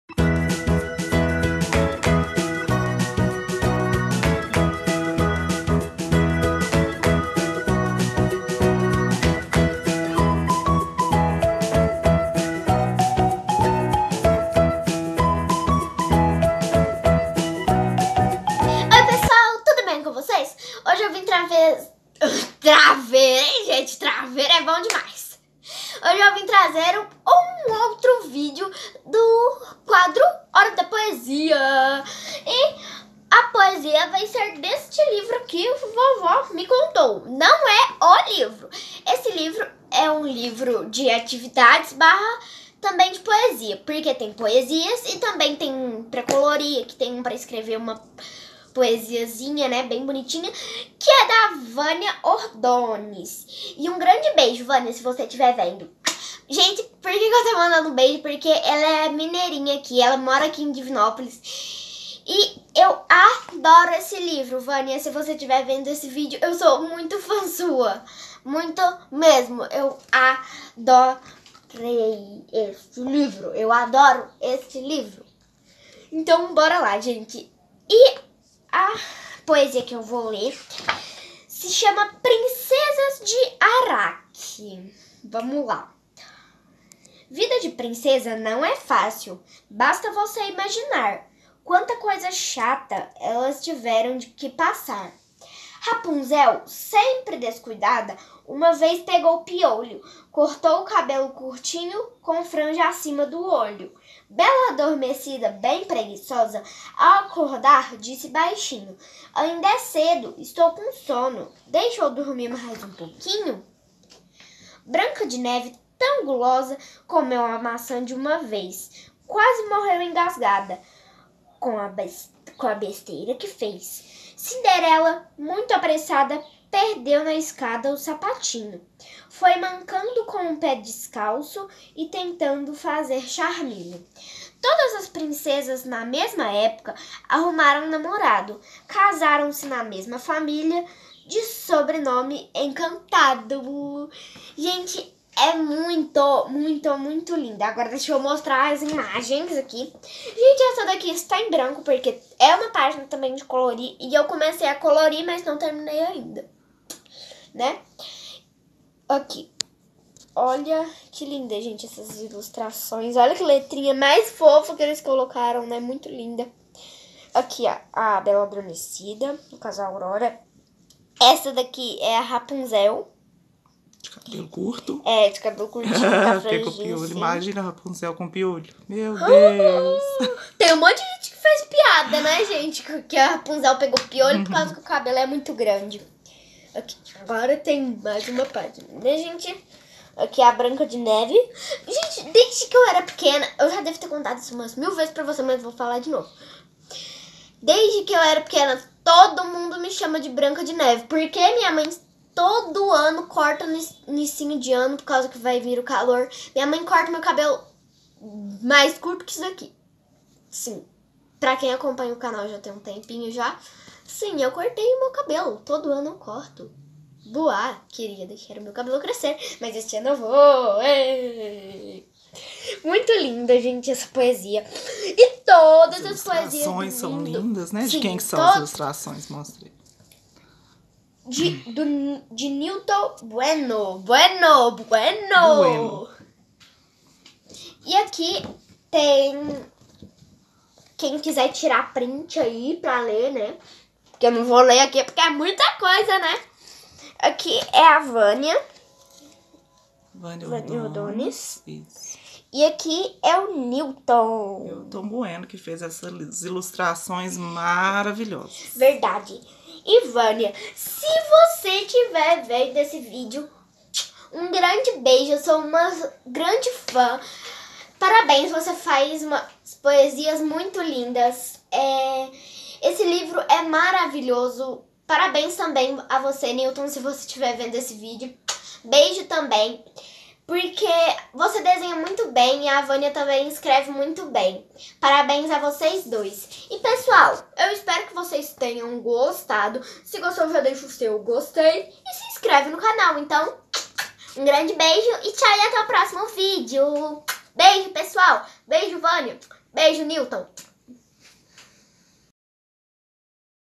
HORA DA POESIA - PRINCESAS DE ARAQUE (VÂNIA ORDONES)
Poesia de hoje: PRINCESAS DE ARAQUE
Do livro: VOVÓ ME CONTOU E OUTRAS MEMÓRIAS
Autora: Vânia Ordones
Ilustrações: Nilton Bueno

Minha Lista de Desejos na Amazon

CANAL TOTALMENTE MONITORADO E SUPERVISIONADO PELA MAMÃE DA BEATRIZ.